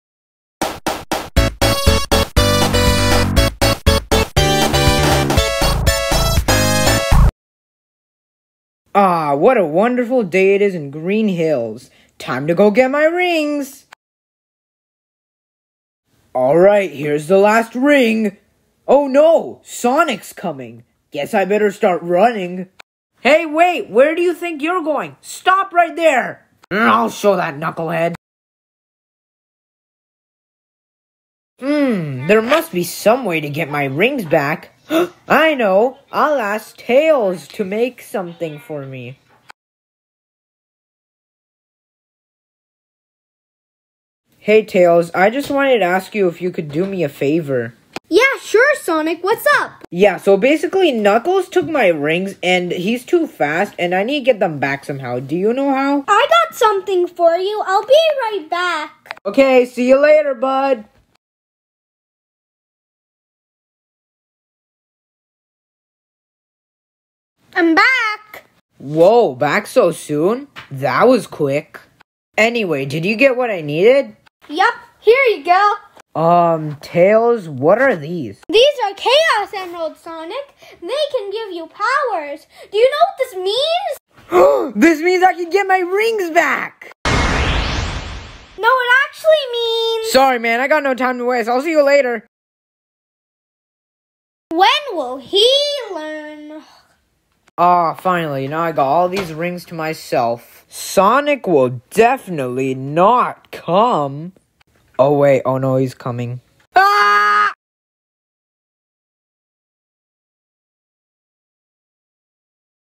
Knuckles steals sonic’s rings, he instantly regrets it….
Fun fact: this is actually a drama presentation for my class

Also thumbnail inspiration is from dhar mann